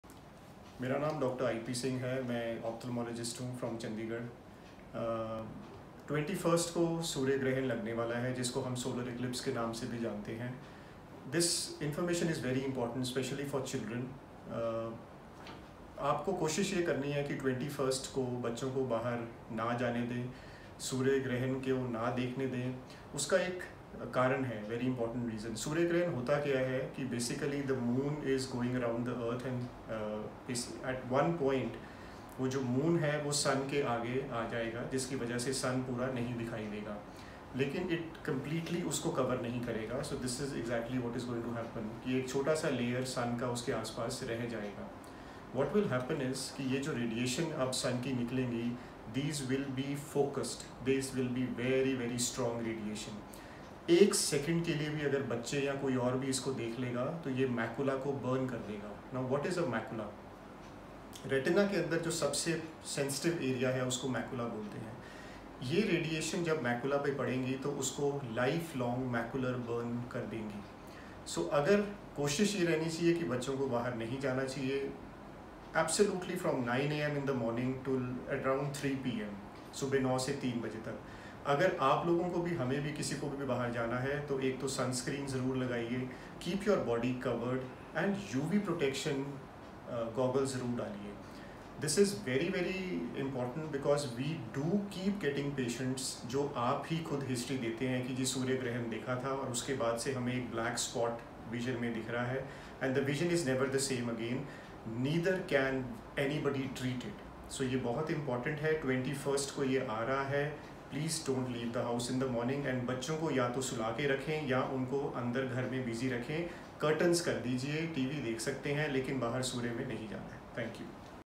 Surya grahan : Solar Eclipse can cause Blindness !!
Coming up Solar eclipse in Asia ..India / Pakistan country’s.